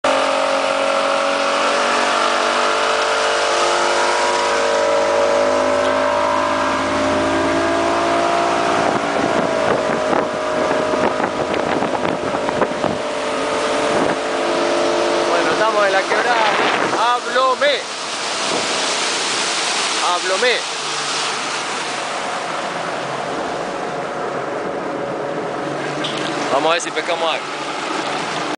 EN LANCHA POR CABRA CORRAL QUEBRADA DE ABLOME DECLARADO INTERES CULTURAL
EN LANCHA POR CABRA CORRAL QUEBRADA DE ABLOME-QUEBRADA DE ABLOMÉ DECLARADA DE INTERÉS CULTURAL Y ... de Turismo de la Provincia y Museo de Antropología de Salta.